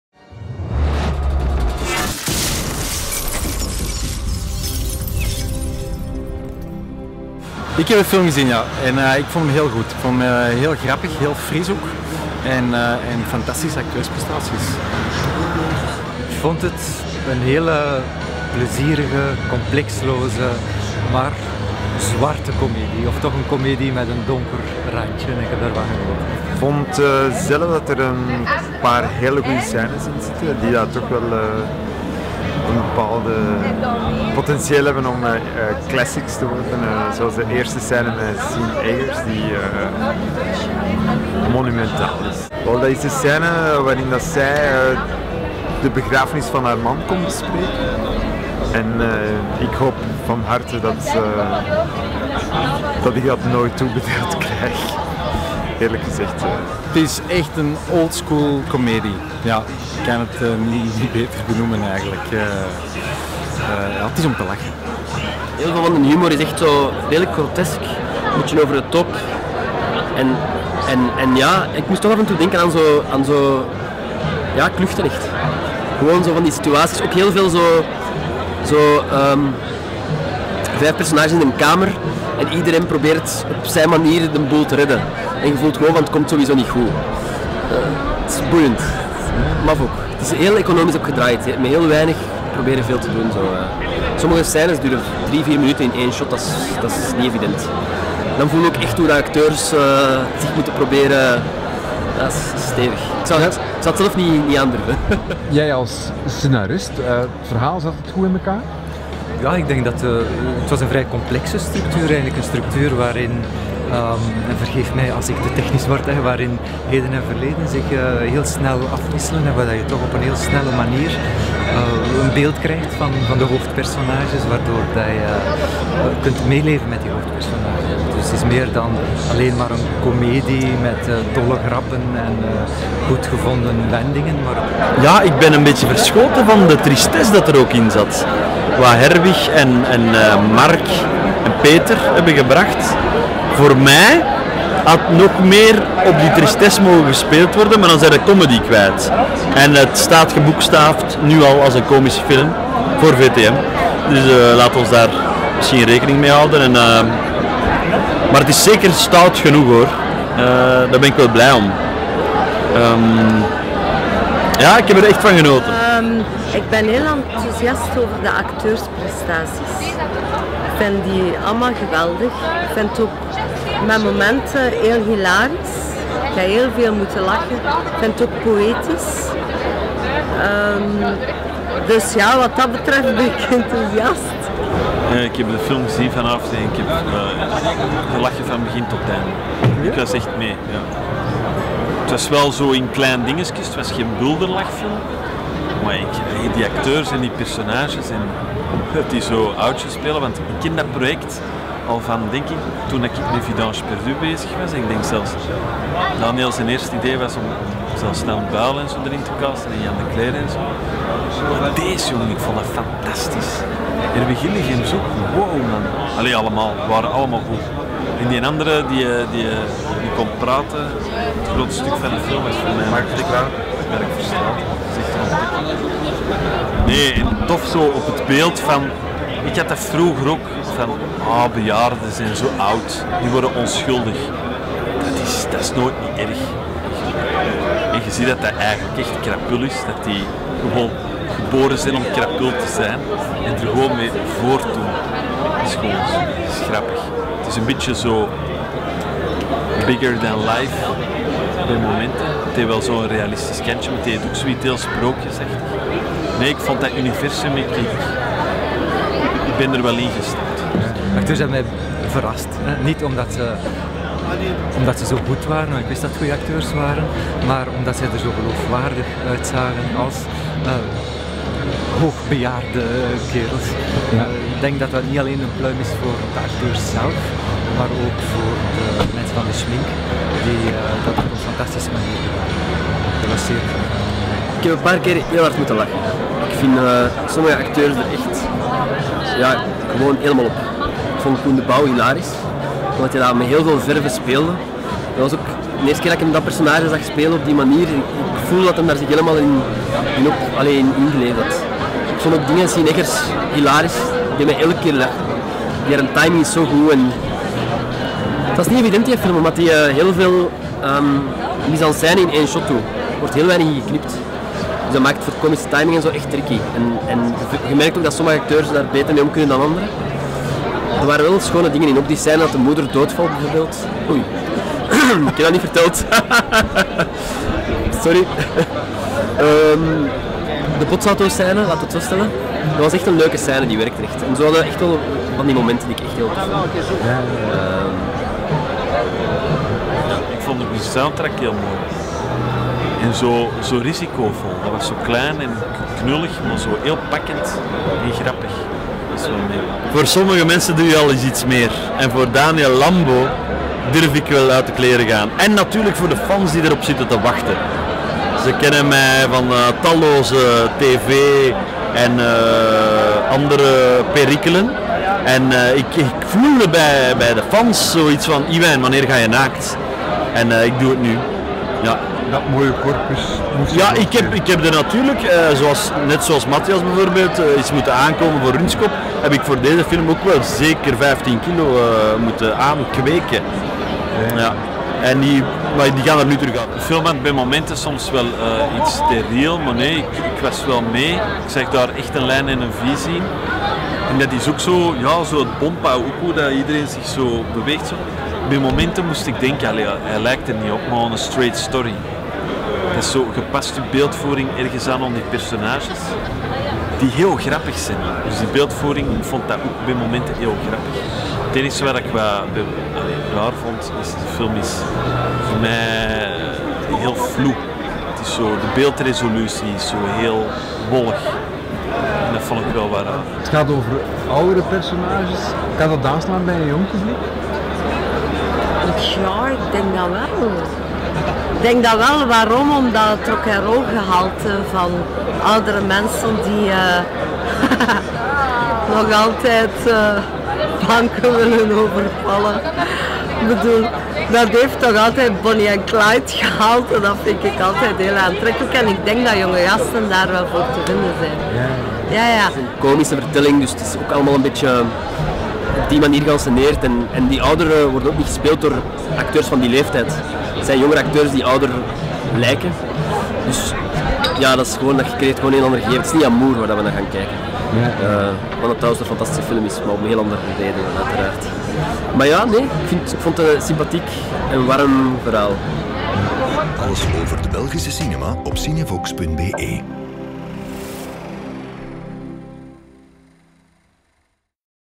Reacties na Los Flamencos
Vrijdagavond 6 september ging in Kinepolis Oostende het 7de Filmfestival van Oostende van start.  Nogal wat namen hadden het laten afweten -- de Rode Duivels op tv, Memorial Vandamme in Brussel en hele zware verkeersproblemen vanuit de richting van Antwerpen -- maar er was nog een heel pak interessant volk. Wij voelden na afloop van de film enkele prominente aanwezigen aan de tand.  Kijk op en u komt nog veel meer te weten over 'Los Flamencos' en over het Filmfestival van Oostende.  En over de Vlaamse film in het algemeen.